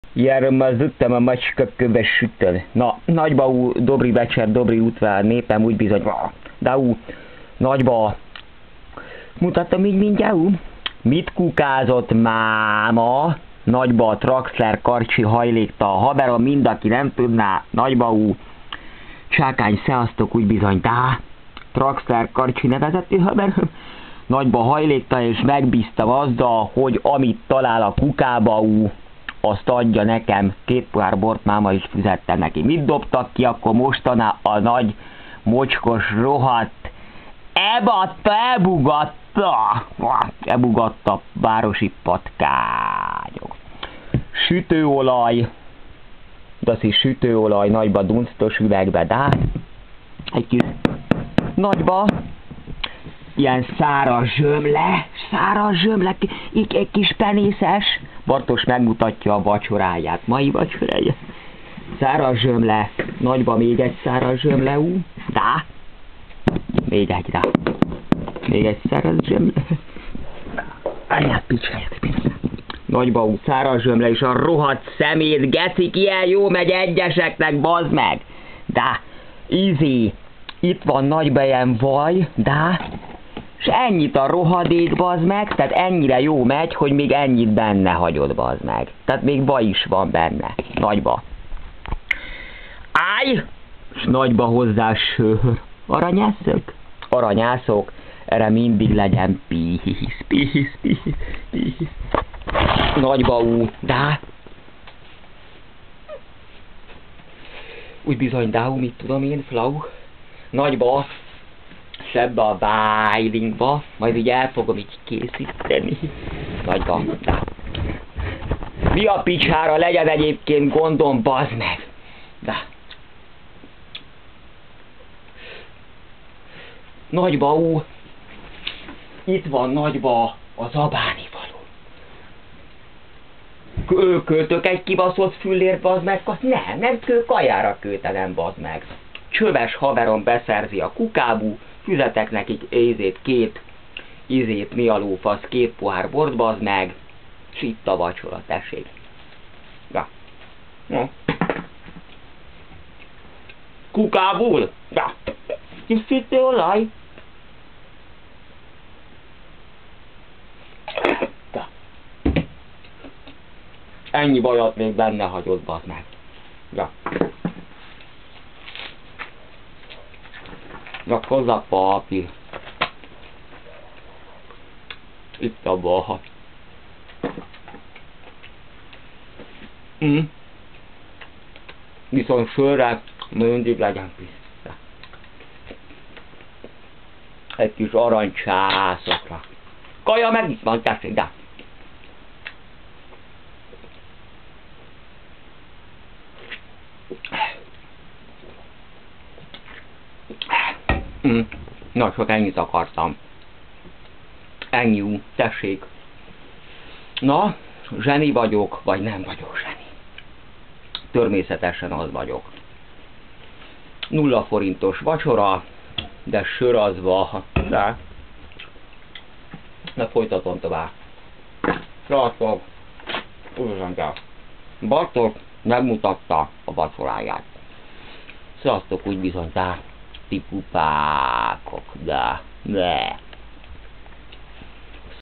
Bartos megmutatja a vacsoráját
Bartos ingyen vacsorája sörrel leöblítve